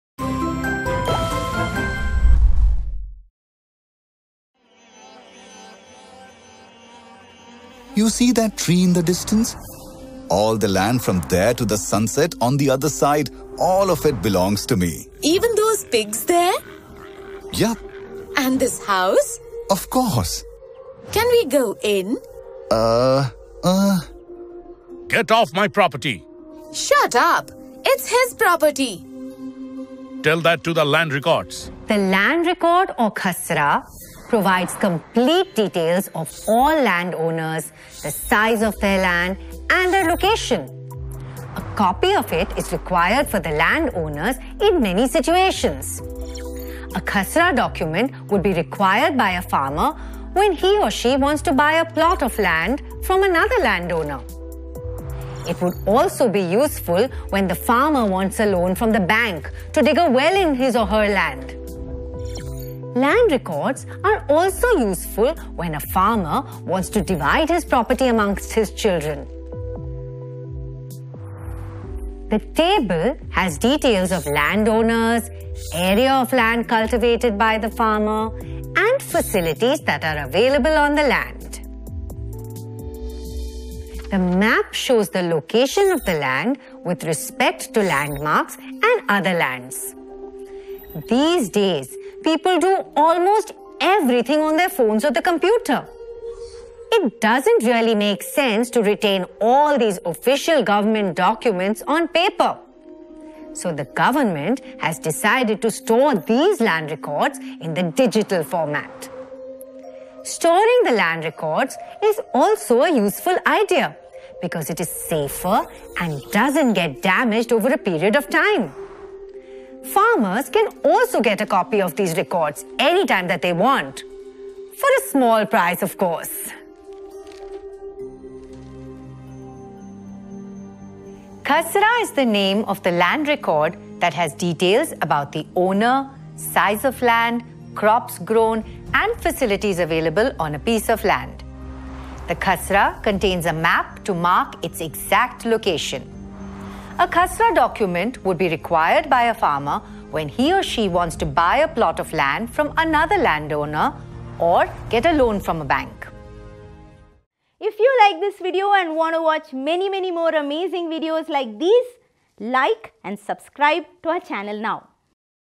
Importance Of A Land Record | Class 6 - Civics | Learn With BYJU'S
Land records are given to the owner of the land as proof that includes the information of the owner, the location and the size of their land. help in administration. Land records are important as it helps in calculating revenue. Watch this video to learn more about land records and its importance.